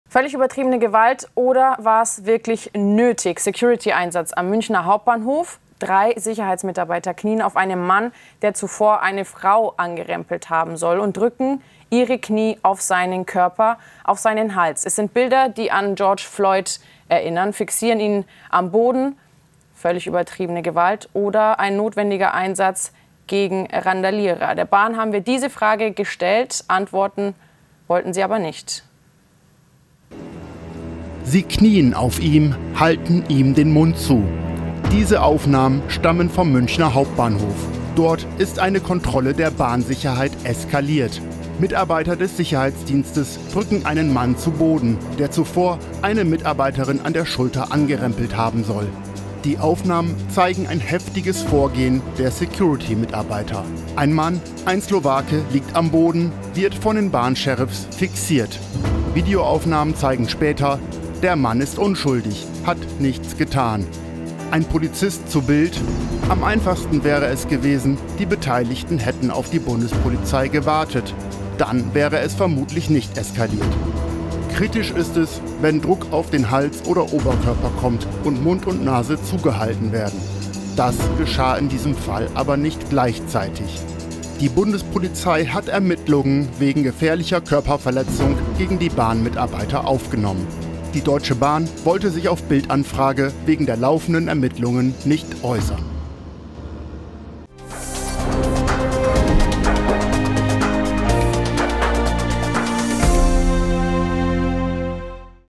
Bilder wie bei George Floyd: Bahn-Security kniet auf Unschuldigen | München
Sie knien auf ihm, halten ihm den Mund zu. Die Bilder erinnern fatal an den Fall des US-Amerikaners ­George Floyd, der starb, nachdem ein Polizist auf seinem Hals kniete.

Doch diese Aufnahmen stammen vom Münchner Hauptbahnhof. Dort ist am Samstag eine Kontrolle eskaliert. Aufnahmen zeigen ein heftiges Vorgehen der Bahn-Sicherheit.

Angeblich soll ein Slowake (28) eine Mitarbeiterin der Bahn am Hauptbahnhof an der Schulter angerempelt haben. Die Frau alarmierte ihre Kollegen der Bahn-Sicherheit.

Als sie den 28-Jährigen gegen 20.30 Uhr an Gleis 22 kontrollieren wollten, ging der aber einfach weiter, war sich keiner Schuld bewusst. Videoaufnahmen belegen: Er hatte nichts getan.